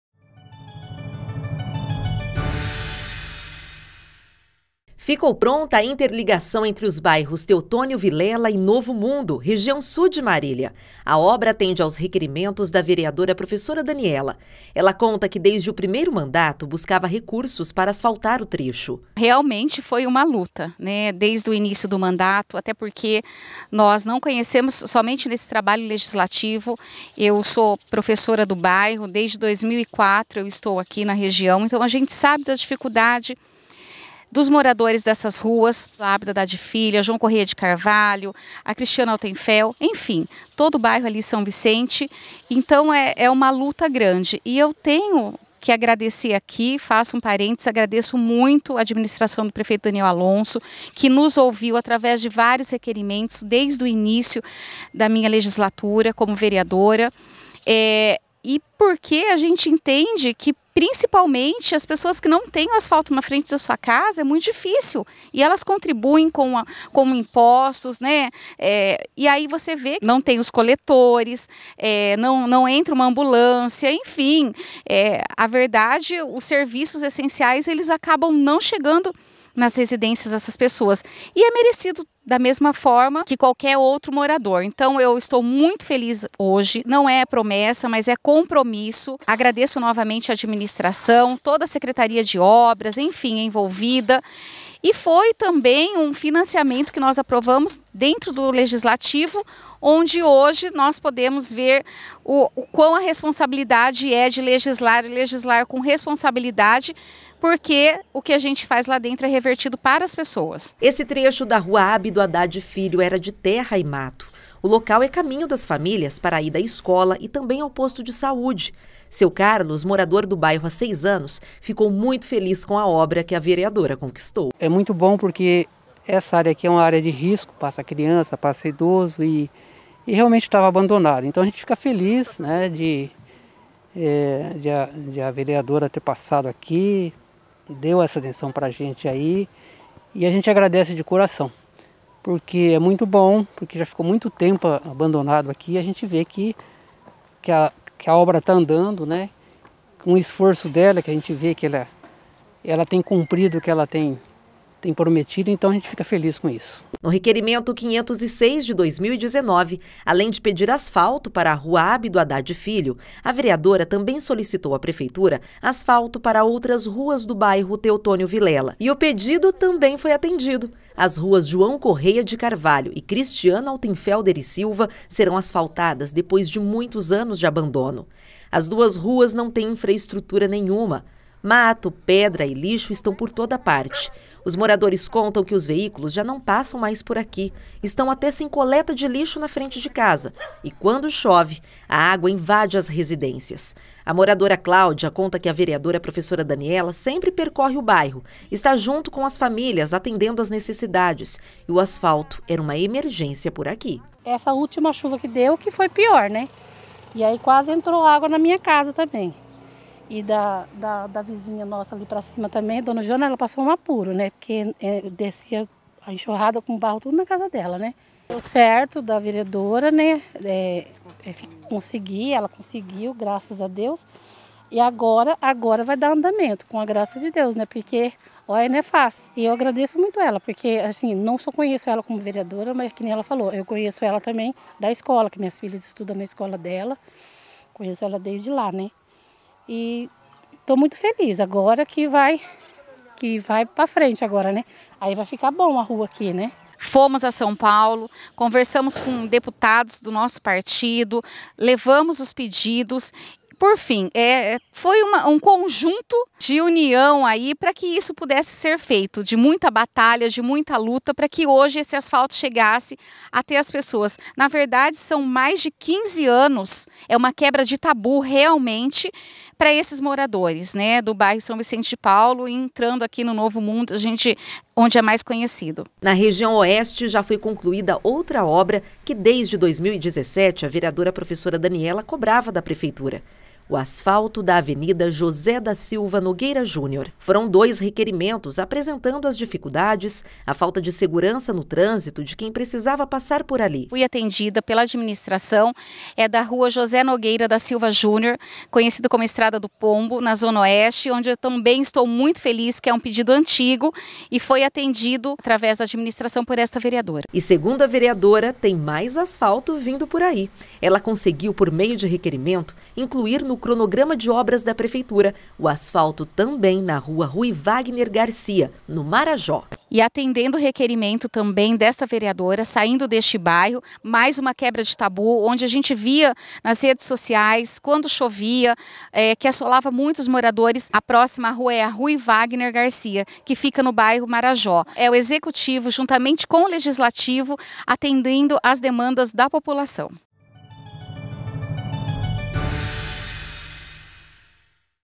VEREADORA PROFESSORA DANIELA CONQUISTA ASFALTO PARA RUAS DA REGIÃO SUL E OESTE
A vereadora Professora Daniela, por meio de requerimentos, conquistou asfalto para as ruas: Abdo Haddad Filho, João Correia de Carvalho, Cristiano Altenfelder e Silva, Ruy Wagner Garcia e Avenida José da Silva Nogueira Júnior.